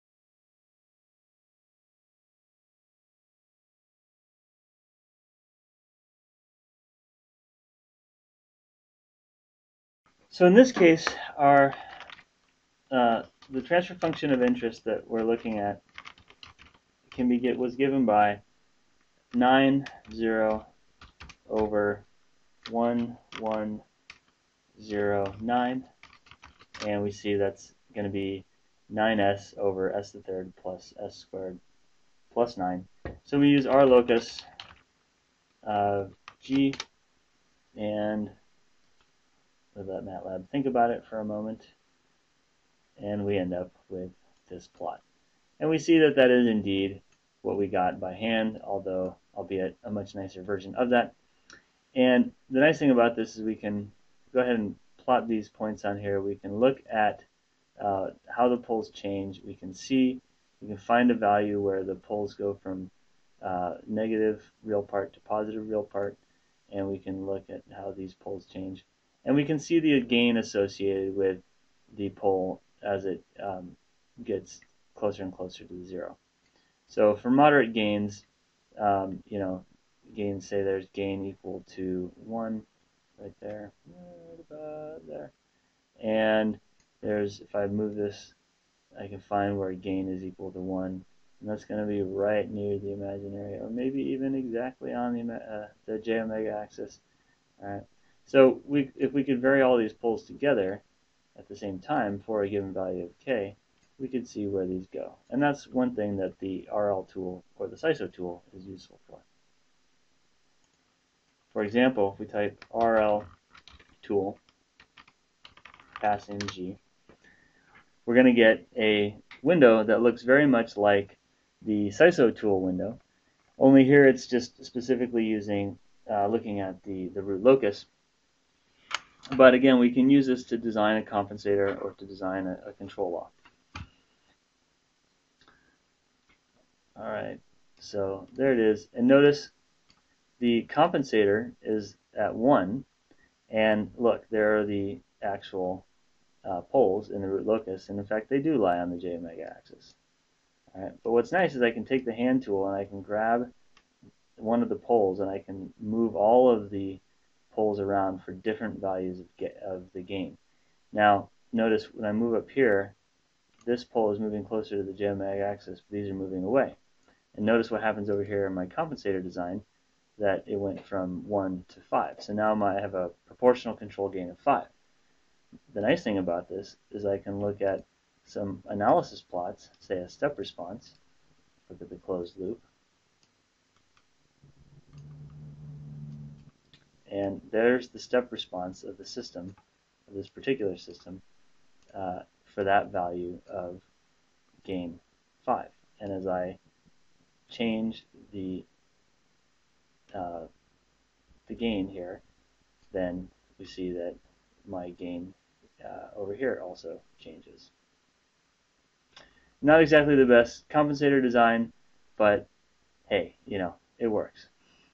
Constructing a Root Locus - Matlab Example
A simple lead filter compensator for a 1-s^2 plant